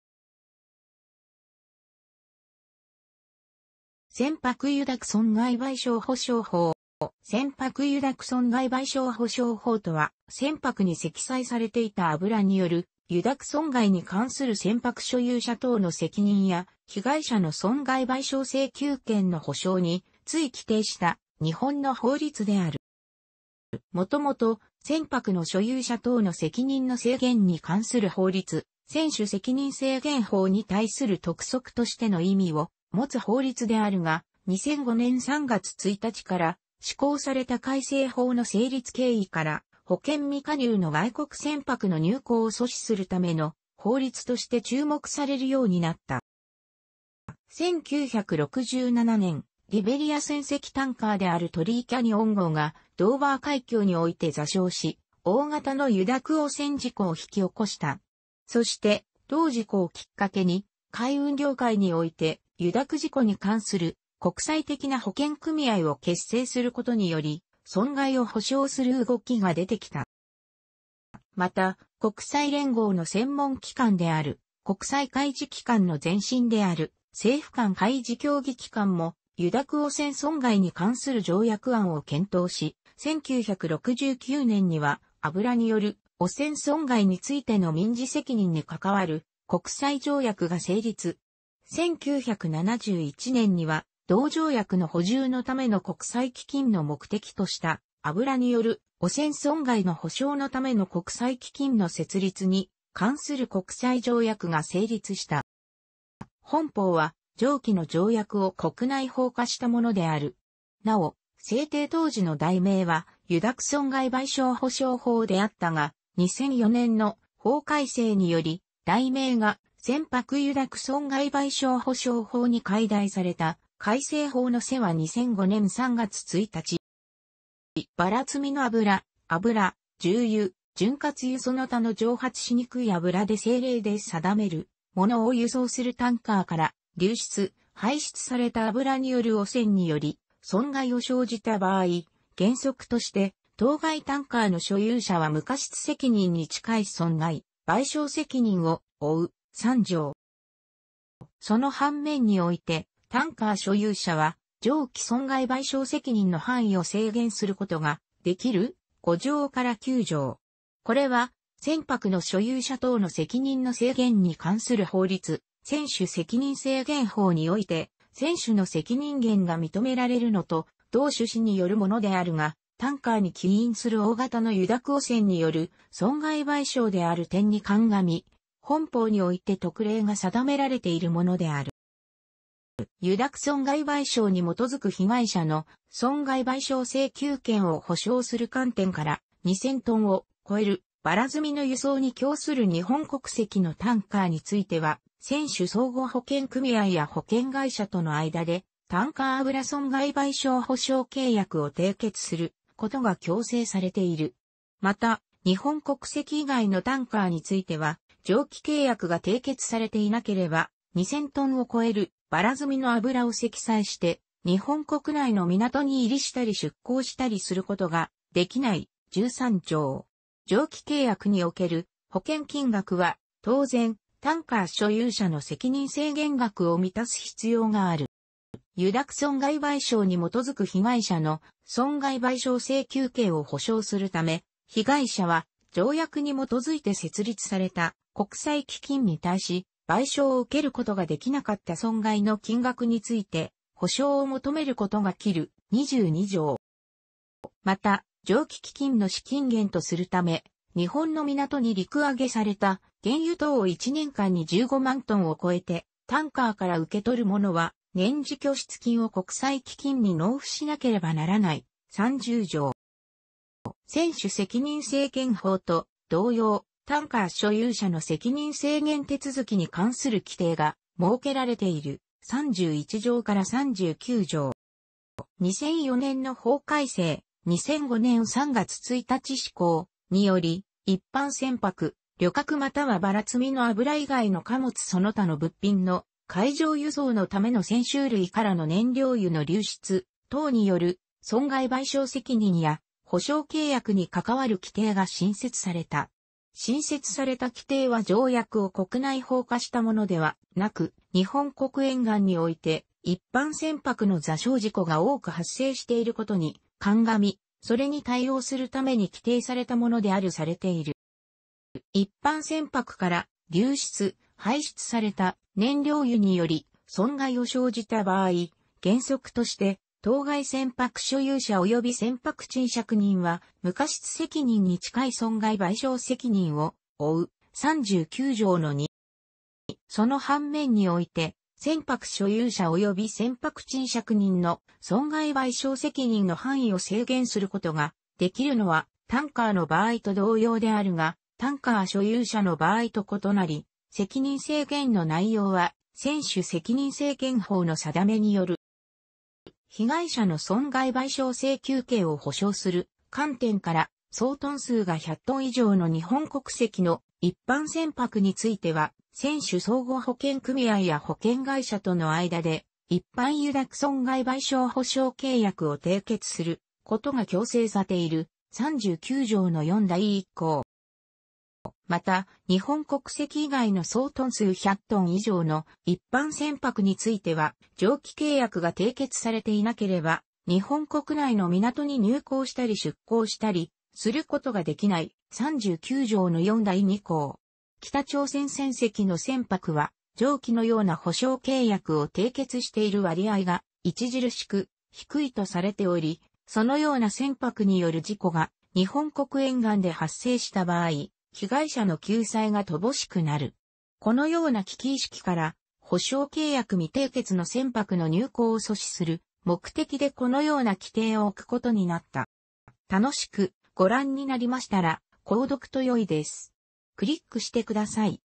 船舶油濁損害賠償保障法
船舶油濁損害賠償保障法（せんぱくゆだくそんがいばいしょうほしょうほう）とは、船舶に積載されていた油による油濁損害に関する船舶所有者等の責任や被害者の損害賠償請求権の保障について規定した日本の法律である。

もともと、船舶の所有者等の責任の制限に関する法律（船主責任制限法）に対する特則としての意味を持つ法律であるが、2005年3月1日から施行された改正法の成立経緯から、保険未加入の外国船舶の入港を阻止するための法律として注目されるようになった。

1967年、リベリア船籍タンカーであるトリー・キャニオン号がドーバー海峡において座礁し、大型の油濁汚染事故を引き起こした。そして、同事故を切っ掛けに、海運業界において油濁事故に関する国際的な保険組合を結成することにより損害を補償する動きが出てきた。

また、国際連合の専門機関である国際海事機関 (IMO) の前身である政府間海事協議機関 (IMCO) も油濁汚染損害に関する条約案を検討し、1969年には「油による汚染損害についての民事責任に関する国際条約」が成立。1971年には、同条約の補充のための国際基金の目的とした「油による汚染損害の補償のための国際基金の設立に関する国際条約」が成立した。

本法は上記の条約を国内法化したものである。なお、制定当時の題名は「油濁損害賠償保障法」であったが、2004年の法改正により題名が「船舶油濁損害賠償保障法」に改題された（改正法の施行は2005年3月1日）。

ばら積みの油（油、重油、潤滑油その他の蒸発しにくい油で政令で定めるもの）を輸送するタンカーから流出・排出された油による汚染により損害を生じた場合、原則として、当該タンカーの所有者は無過失責任に近い損害賠償責任を負う（3条）。

その反面において、タンカー所有者は上記損害賠償責任の範囲を制限することができる（5条〜9条）。これは、船舶の所有者等の責任の制限に関する法律（船主責任制限法）において船主の責任制限が認められるのと同趣旨によるものであるが、タンカーに起因する大型の油濁汚染による損害賠償である点に鑑み、本法において特例が定められているものである。

油濁損害賠償に基づく被害者の損害賠償請求権を保障する観点から、2000トンを超えるばら積みの輸送に供する日本国籍のタンカーについては、船主相互保険組合や保険会社との間でタンカー油濁損害賠償保障契約を締結することが強制されている。また、日本国籍以外のタンカーについては、上記契約が締結されていなければ、2000トンを超えるばら積みの油を積載して日本国内の港に入港したり出港したりすることができない（13条）。上記契約における保険金額は、当然、タンカー所有者の責任制限額を満たす必要がある。

油濁損害賠償に基づく被害者の損害賠償請求権を保障するため、被害者は、条約に基づいて設立された国際基金に対し、賠償を受けることができなかった損害の金額について補償を求めることができる（22条）。

また、上記基金の資金源とするため、日本の港に陸揚げされた原油等を1年間に15万トンを超えてタンカーから受け取る者は、年次拠出金を国際基金に納付しなければならない（30条）。

船主責任制限法と同様、タンカー所有者の責任制限手続に関する規定が設けられている（31条〜39条）。

2004年の法改正（2005年3月1日施行）により、一般船舶（旅客又はばら積みの油以外の貨物その他の物品の海上輸送のための船舟類）からの燃料油の流出等による損害賠償責任や保障契約に関する規定が新設された。新設された規定は条約を国内法化したものではなく、日本国沿岸において一般船舶の座礁事故が多く発生していることに鑑み、それに対応するために規定されたものであるとされている。

一般船舶から流出・排出された燃料油により損害を生じた場合、原則として当該船舶所有者及び船舶賃借人は、無過失責任に近い損害賠償責任を負う（39条の2）。

その反面において、船舶所有者及び船舶賃借人の損害賠償責任の範囲を制限することができるのはタンカーの場合と同様であるが、タンカー所有者の場合と異なり、責任制限の内容は船主責任制限法の定めによる。

被害者の損害賠償請求権を保障する観点から、総トン数が100トン以上の日本国籍の一般船舶については、船主相互保険組合や保険会社との間で一般油濁損害賠償保障契約を締結することが強制されている（39条の4第1項）。

また、日本...